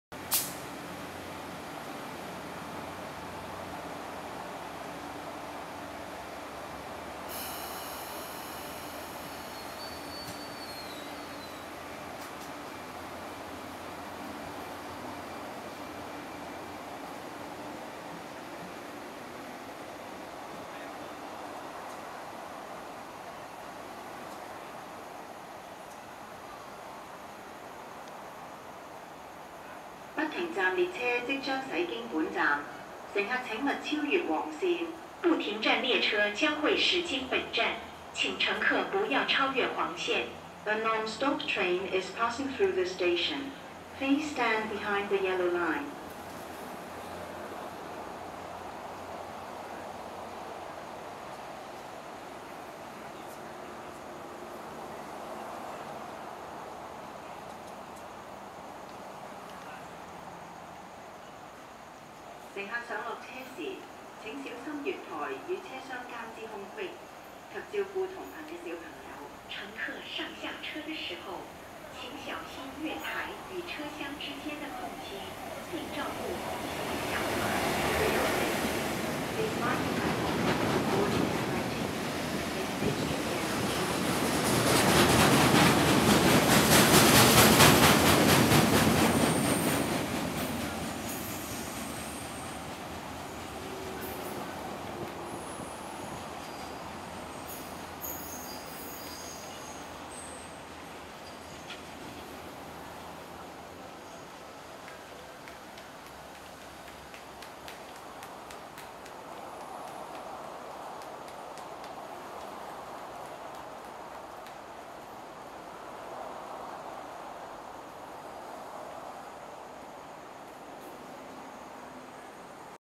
The Southbound freight train single head GM EMD G16 58 Gordon Graham
The Southbound freight train single head GM EMD G16 58 Gordon Graham haul the wagons and Box car pass the Sha Tin Station.

Time: pm
Date:08/06/2008
Canon G7 30 fps 640x480 standard
Place: Sha Tin Station
Software: Apple QuickTime Pro 7.5.5 (AVI to MOV)